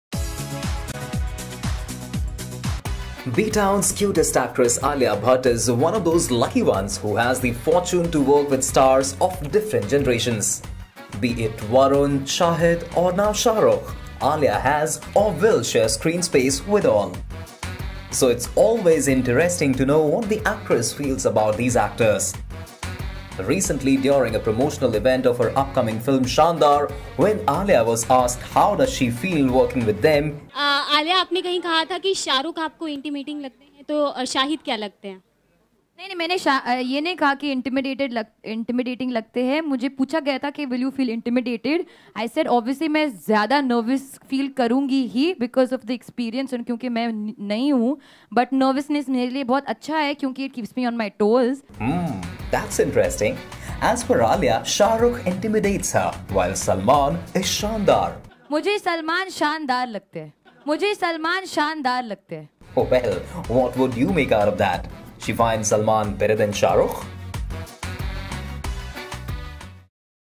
Alia Compares Salman & Shahrukh | SHAANDAAR
Recently during a promotional event of her upcoming film 'Shaandaar', Alia Bhatt opened up about Bollywood's two biggest superstars, Salman Khan and Shahrukh Khan. To know more, watch the story.